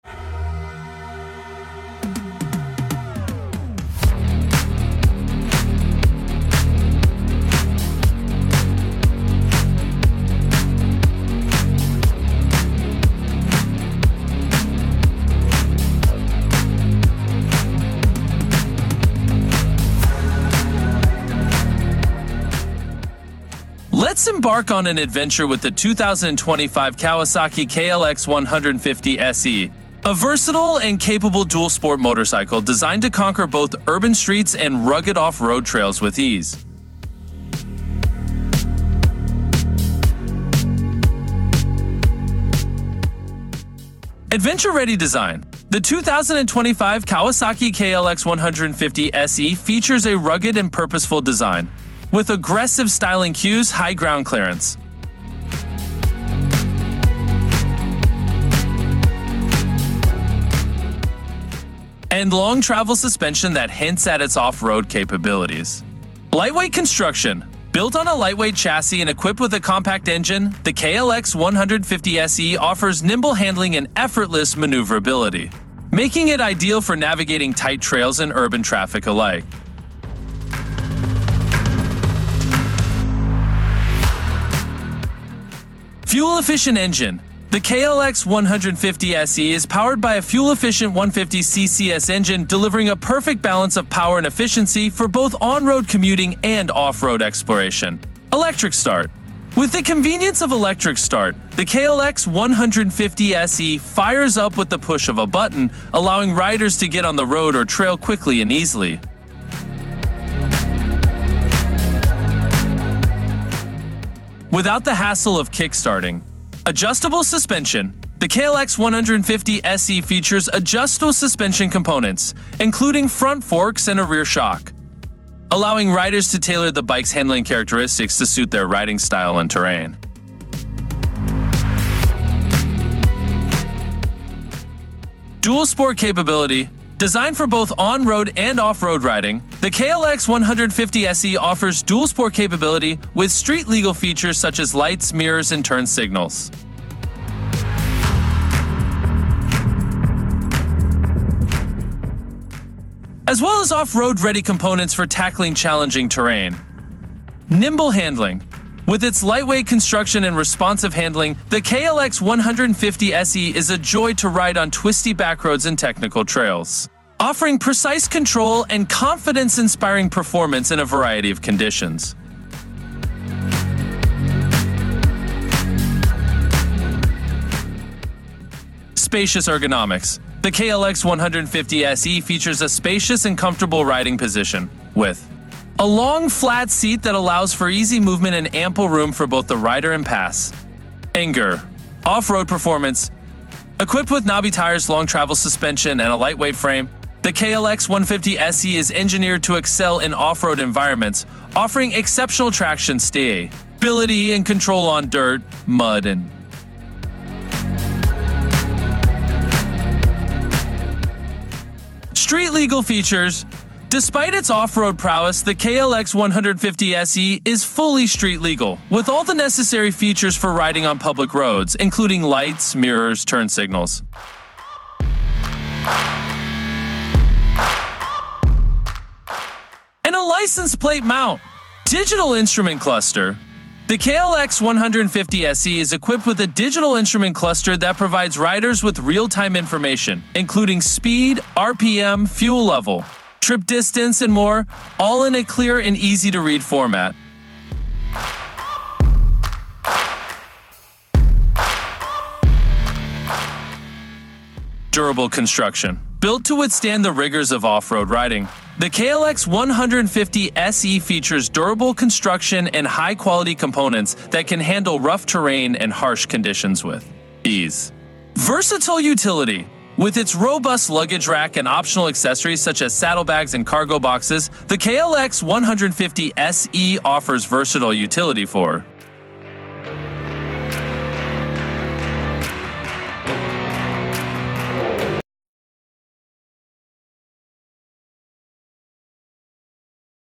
2025 Kawasaki KLX150SE Unveiled
2025 Kawasaki KLX150SE Unveiled @motoadv6305
The Kawasaki KLX150SE is typically known as a lightweight dual-sport motorcycle, designed for both on-road and off-road adventures. Here are some aspects you might expect:

Engine and Performance: The KLX150SE usually features a modest yet reliable engine, often around 144cc, designed for easy handling and decent off-road capability without being too powerful for beginners.

Chassis and Suspension: It typically comes with a sturdy frame suitable for varied terrain, and a suspension setup that can handle bumps and uneven surfaces well, making it versatile for both urban commuting and trail riding.